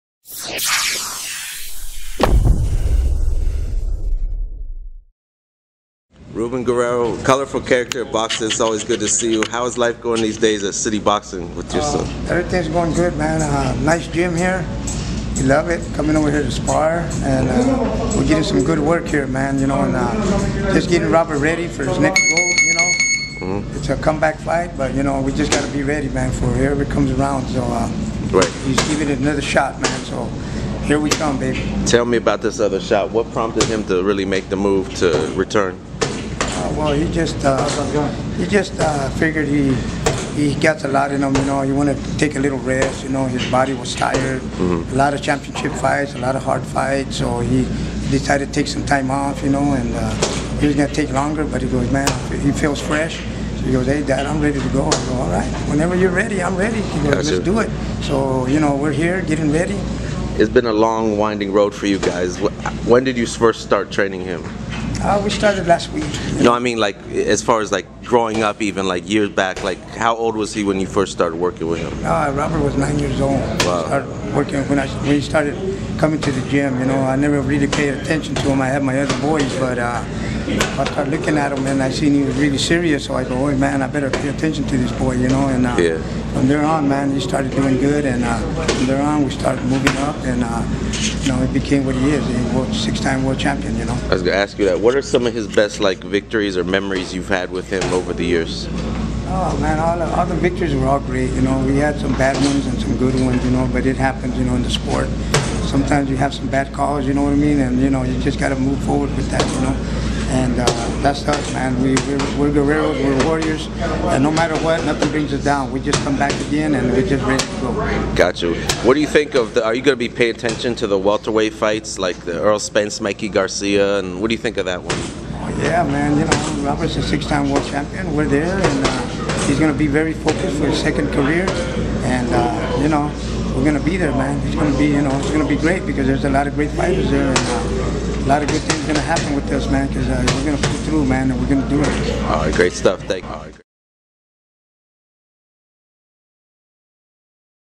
Ruben Guerrero on the comeback of his son Robert 'The Ghost' Guerrero
steps inside of City Boxing Club in Las Vegas, as trainer Ruben Guerrero reflects on the comeback of his son, six-time world champion Robert "The Ghost" Guerrero (33-6-1, 18 KO's).